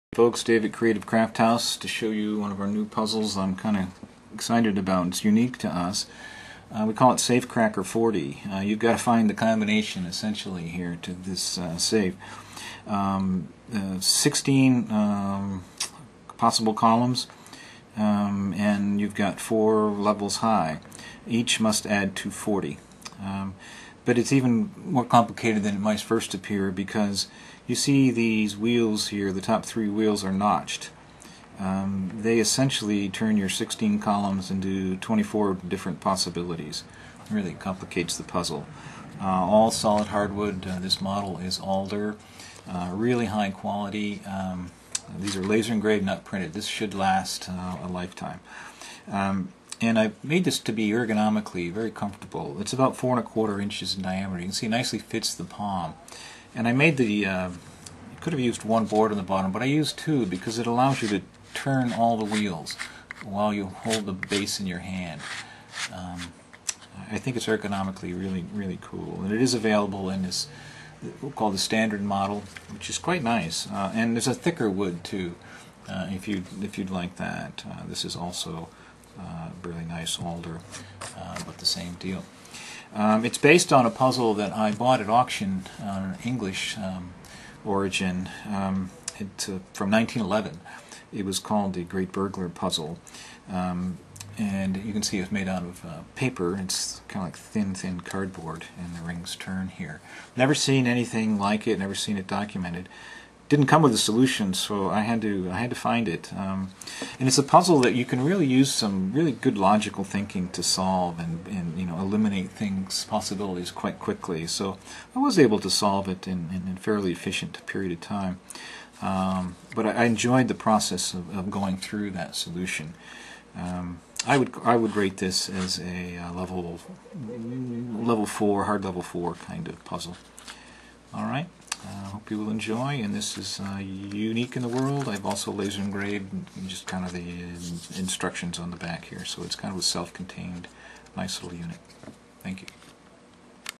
Safecracker 40 math wooden brain teaser puzzle
Unique precision cut puzzle - turn the wheels to get all columns to add to 40.  Only 1 possible solution.